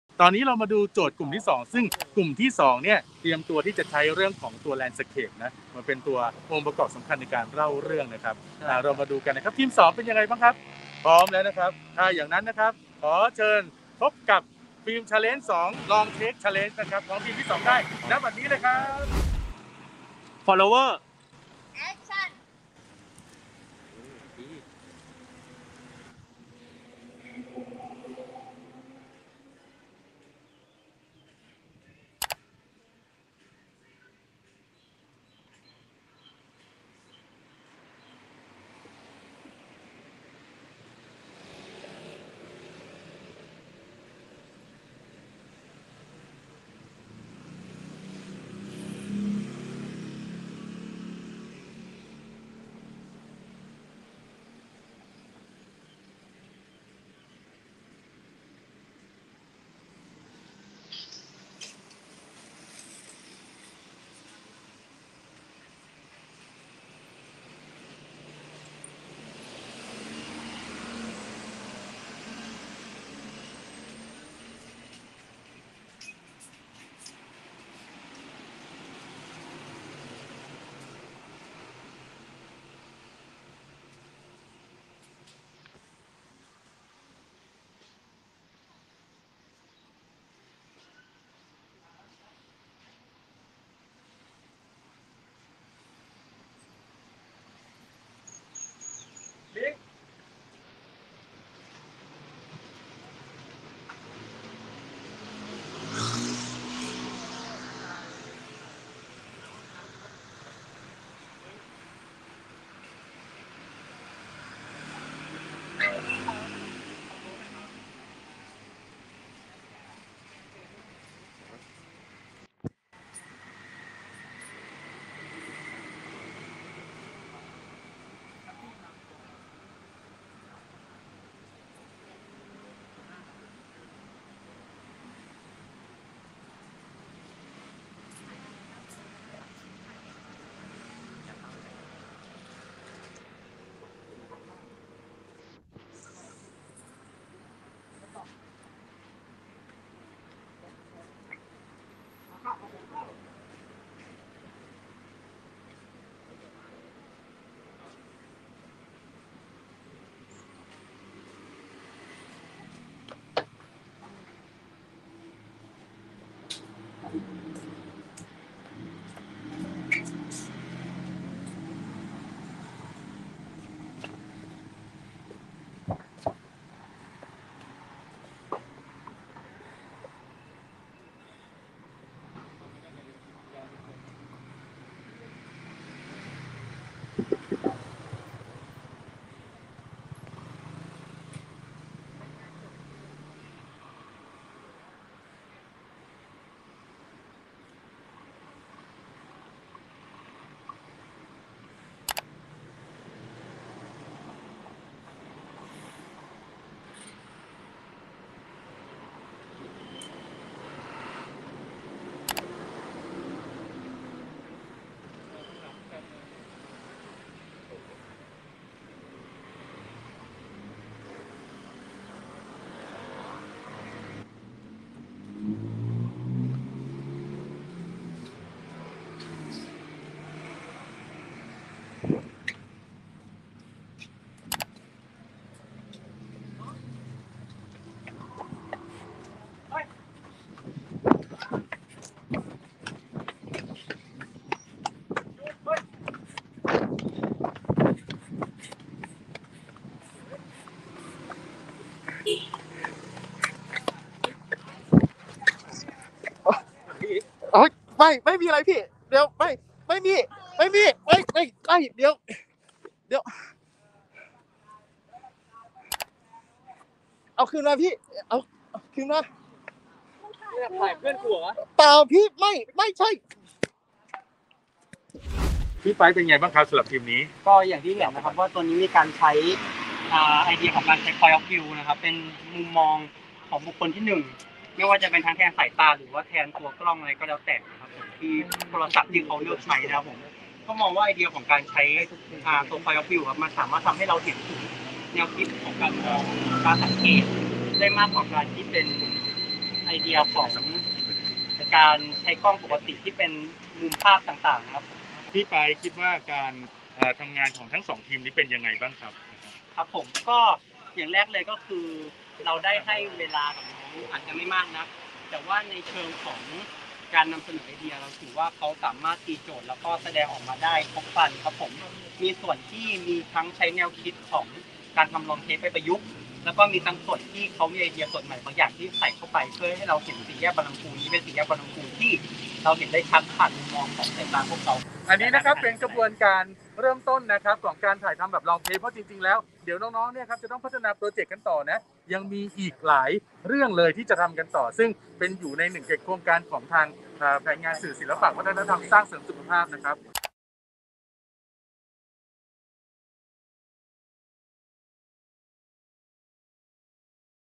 Film Challenge 2 Long Take Story 2
🎬 ปันศิลป์ ปันสุข...Film Challenge 2 Long Take Competition ผลงานเรื่องที่ 2 ของน้องๆ มัธยม ว่าด้วยการใช้ Point of View จะเป็นอย่างไร ติดตามกันได้..
ส่วนใครที่อยากดูเต็มตลอดไลฟ์ สามารถติดตามได้ทาง ⬇️ ตั้งแต่นาทีที่ 25.30 เป็นต้นไป

ติดตามข่าวสารและกิจกรรมปันศิลป์ ปันสุข เพิ่มเติมได้ที่